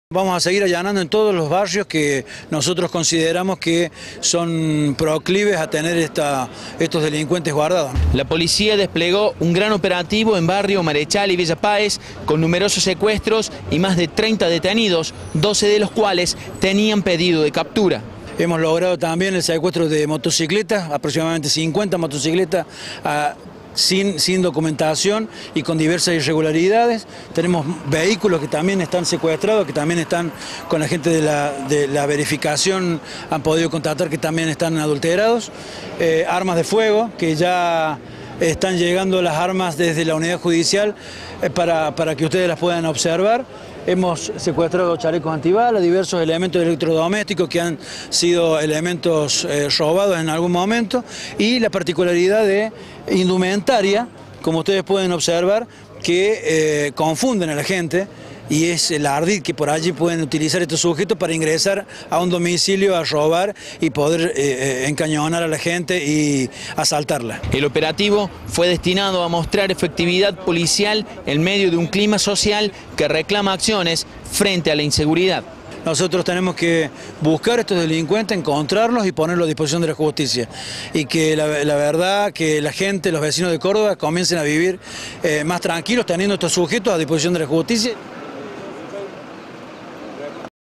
Allanamientos dejan 30 detenidos en Villa Páez y Marechal
En los operativos policiales se secuestraron 50 motocicletas, electrodomésticos, armas de fuego y chalecos antibalas.